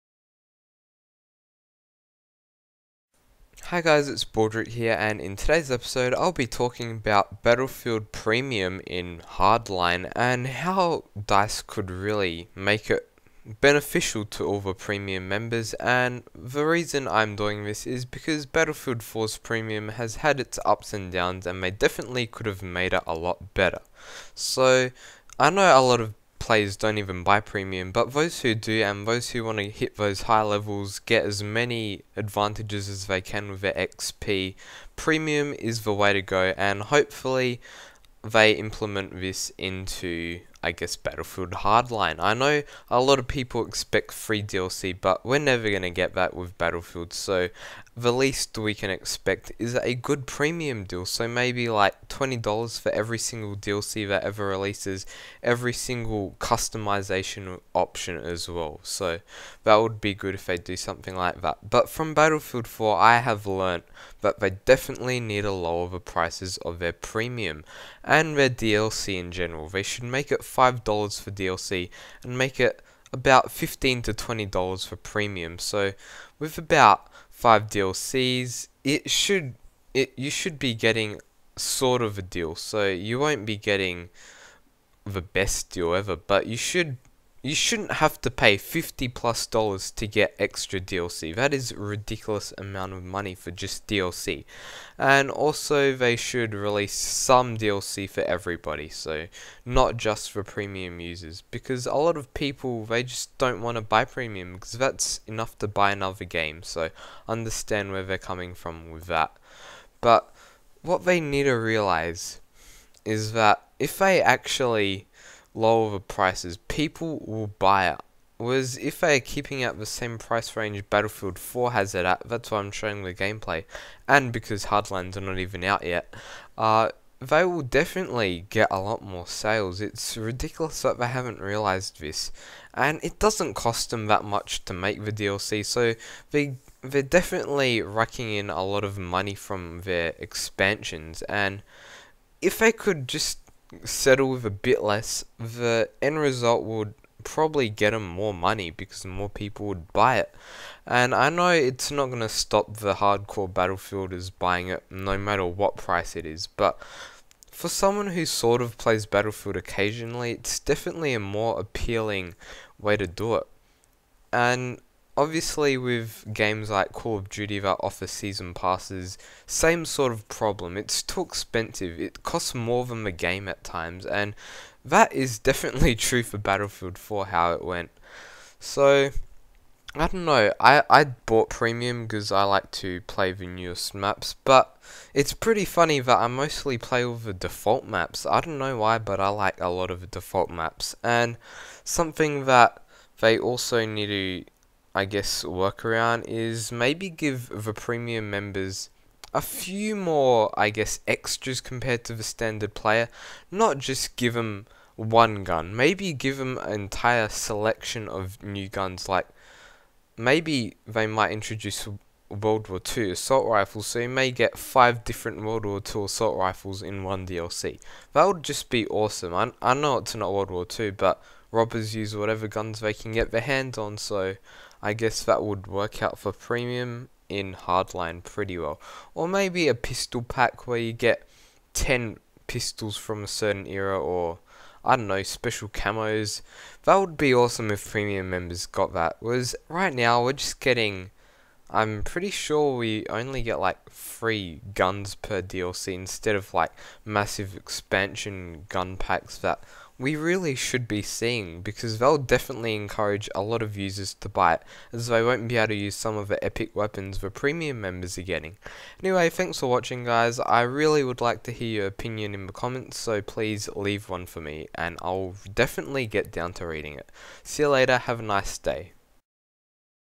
How Battlefield Hardline Should Handle Premium
Hi everyone in todays video I go over what BF Premium is like for BF4 and what aspects DICE should incorporate into the upcoming game Battlefield Hardline.